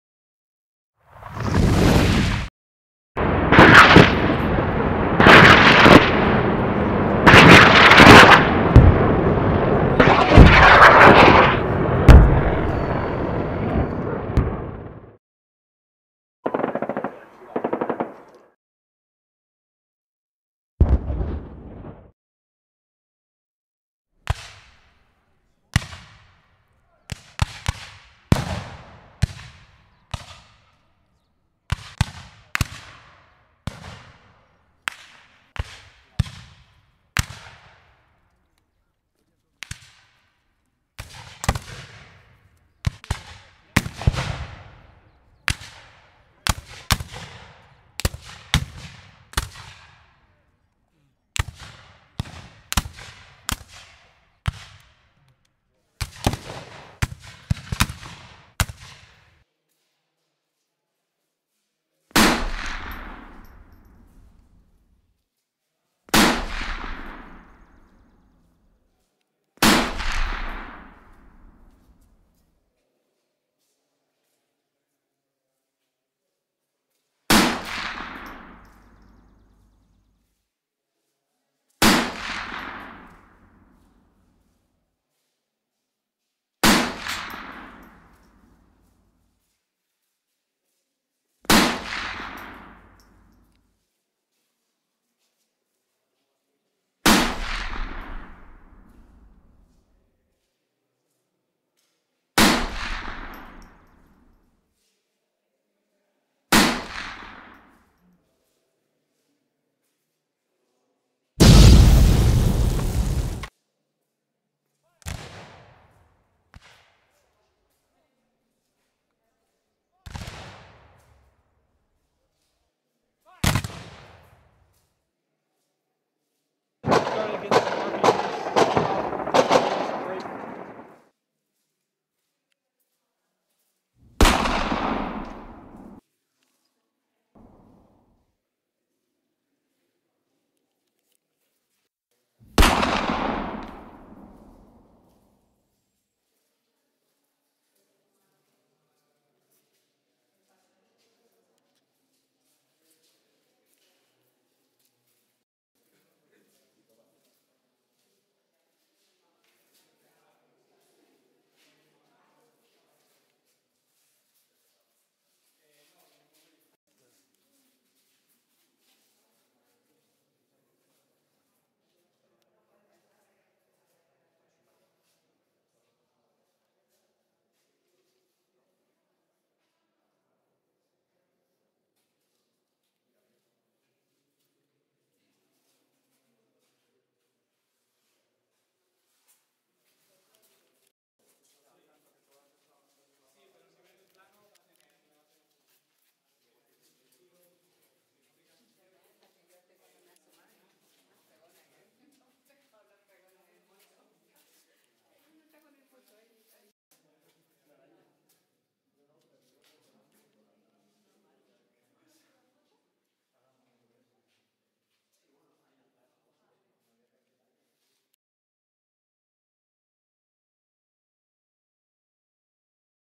La posición Jaca o el búnker de El Capricho (Madrid)
Se construyó en secreto al noreste de Madrid en 1.937, en un lugar estratégico, cerca de las carreteras de Barcelona y Valencia (zona republicana) y cerca de Alcalá de Henares (donde se ubicaba el aeródromo soviético). Fue sede del Estado Mayor del Ejército Republicano.

Está en los jardines de El Capricho, junto al palacio. Sin embargo, no existe ningún túnel que comunique el búnker con el palacio.

La vegetación de los jardines ocultaría el búnker, hecho con ladrillo y hormigón y diseñado para aguantar bombas de hasta 100 kg de peso. Tiene 2000 metros cuadrados, 11 salas y 15 metros de profundidad. En teoría se diseñó para que 200 personas pudieran aguantar dentro 15 días. El pasillo central mide 34 metros de largo y 2 y medio de alto.

Dispone de un generador de gasóleo, varios respiraderos, agua (un aljibe alimentado por un viaje de agua), fosa séptica, y 4 entradas (o salidas).

En su construcción intervinieron ingenieros navales y lo prepararon para resistir ataques con gas. Esto se ve en las puertas, estancas como las de barcos y submarinos.

Los agujeros de las paredes son los de los respiraderos, que se colocaban en la parte inferior de las paredes porque en caso de ataque de gas había que ponerse la máscara antigás y echarse cuerpo a tierra. El aire caliente subía y quemaba los pulmones.

Los patrones del suelo y la cenefa de la pared sirven para orientarse con poca luz.

Los pasillos de entrada forman un ángulo de 90 grados para protegerse de los impactos de la artillería. Luego las puertas cada vez se disponen a un lado y se enfrentan a muros, por el mismo motivo.

El general Miaja (presidente de la Junta de Defensa de Madrid y encargado de organizar la defensa de la ciudad) tuvo su despacho en el palacio y su puesto fue ocupado después por el coronel Casado.

El búnker no sufrió  ningún ataque por parte del otro bando, los únicos combates que presenció fueron entre partidarios y detractores de Casado al final de la guerra.

En la sala del generador se pintaron las paredes de negro y se rodó en los años 70 la película "El gran amor del conde Drácula".

Una bomba va sacando continuamente el agua que penetra en el búnker. Al abandonarse, el búnker se inundó por completo y se perdió todo el mobiliario (menos el casquillo de una bombilla).

Efectos de sonido:
Big Explosion Sweeping In
Airplane Rocket Fire Close
Gunfire
Big Explosion Distant
Rifle Musket Fire Random
Long Rifle Shots
Big Explosion Cut Off
Rifle Musket Fire Volley
Gunfire And Voices
Gunfire Long Distance